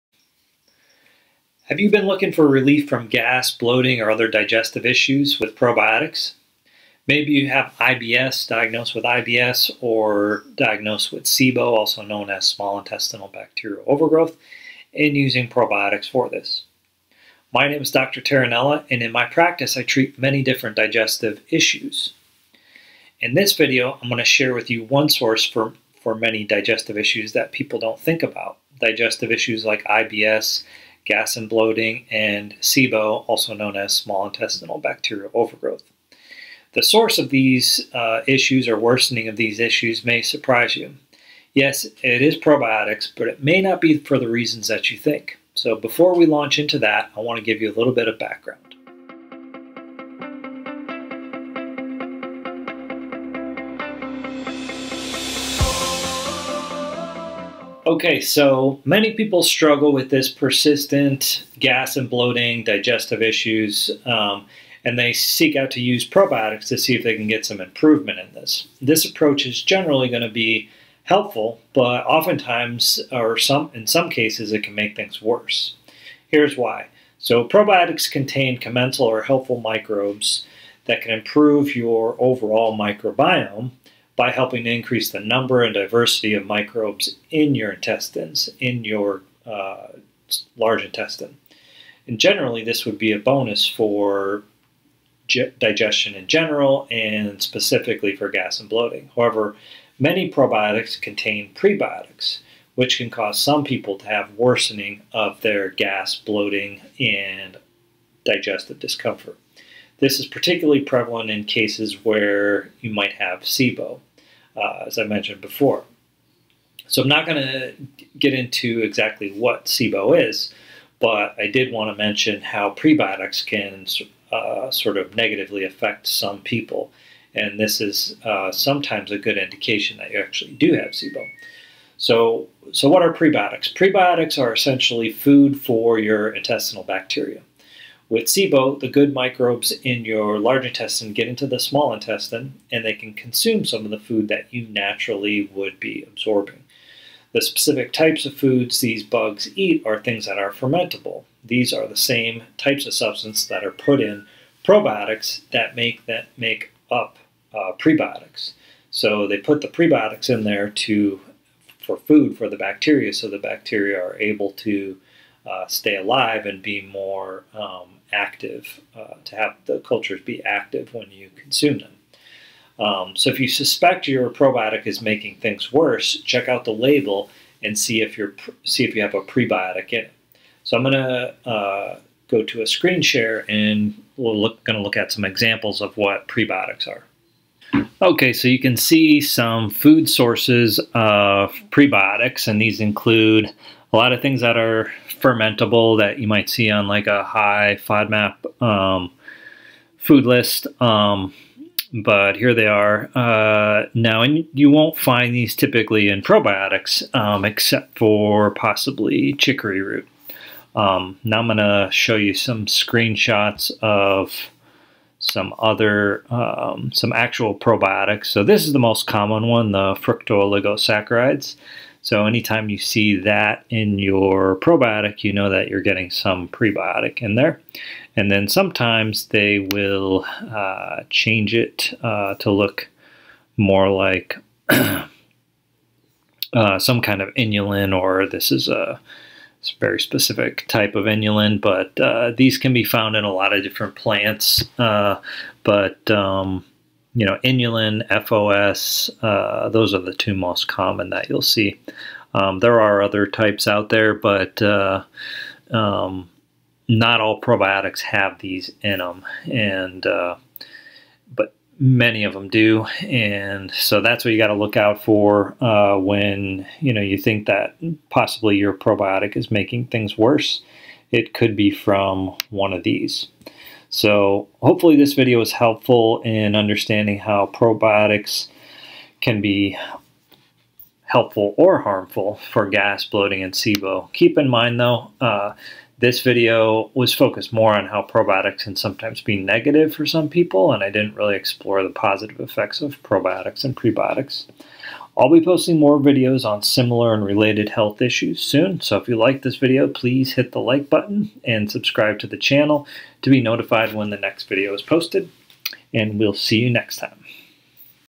SIBO, Probiotics, and Increased Gas?
SIBO probiotics may increase gas and SIBO symptoms. We will discuss why and how probiotics can sometimes have a negative impact on digestion.
have you been looking for relief from gas bloating or other digestive issues with probiotics maybe you have IBS diagnosed with IBS or diagnosed with SIBO also known as small intestinal bacterial overgrowth and using probiotics for this. my name is dr. tarantella and in my practice I treat many different digestive issues. in this video I'm going to share with you one source for for many digestive issues that people don't think about. digestive issues like IBS gas and bloating and SIBO also known as small intestinal bacterial overgrowth. the source of these issues are worsening of these issues may surprise you. yes it is probiotics but it may not be for the reasons that you think so before we launch into that I want to give you a little bit of background.

okay so many people struggle with this persistent gas and bloating digestive issues and they seek out to use probiotics to see if they can get some improvement in this this approach is generally going to be helpful but often times or something in some cases it can make things worse. here's why so probiotics contain commensal or helpful microbes that can improve your overall microbiome by helping to
increase the number and diversity of microbes in your intestines in your large intestine. generally this would be a bonus for digestion in general and specifically for gas and bloating.
however many probiotics contain prebiotics which can cause some people to have worsening of their gas bloating and digestive discomfort. this is particularly prevalent in cases where you might have SIBO as I mentioned before. so I'm not gonna get into exactly what SIBO is but I did want to mention how prebiotics can sort of negatively affect some people and this is sometimes a good indication they actually do not see them. so so what are prebiotics ?probiotics are essentially food for your intestinal bacteria. with SIBO the good microbes in your large intestine get into the small intestine and they can consume some of the food that you naturally would be absorbing the specific types of foods these bugs eat are things that are fermentable. these are the same types of substance that are put in probiotics that make that make up prebiotics. so they put the prebiotics in there too for food for the bacteria so the bacteria are able to stay alive and be more active to have the cultures be active when you consume them. so if you suspect your probiotic is making things worse check out the label and see if they have a prebiotic in it.